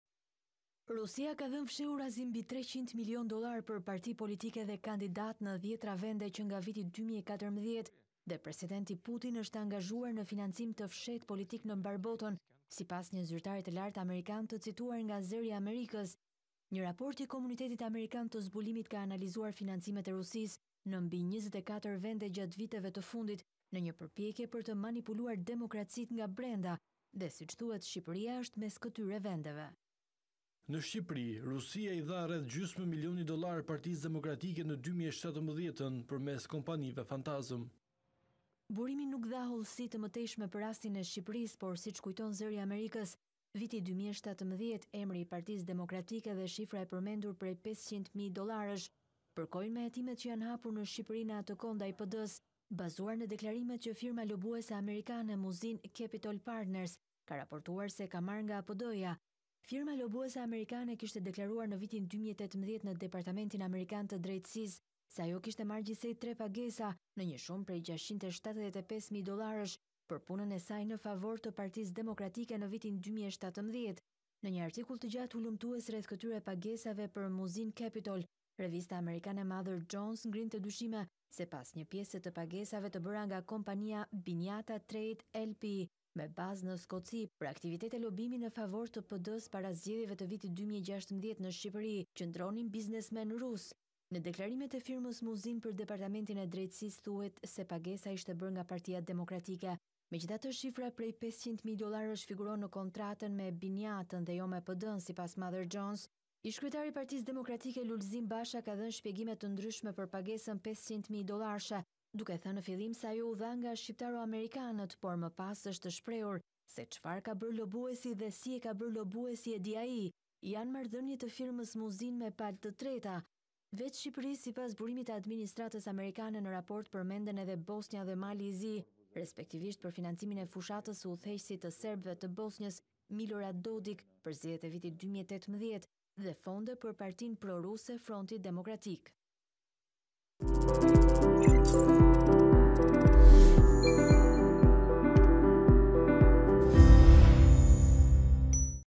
Zbulimi i inteligjencës amerikane, Putin i dha 500 mijë dollarë PD-së drejtuar nga Basha
Rusia ka financuar fshehurazi parti politike në dhjetëra vende, mes të cilave edhe Partinë Demokratike në Shqipëri. Sipas inteligjencës amerikane PD-ja e drejtuar nga Lulzim Basha në 2017-tën,  ka marrë 500 mijë dollarë nga Moska, përmes kompanive fantazmë.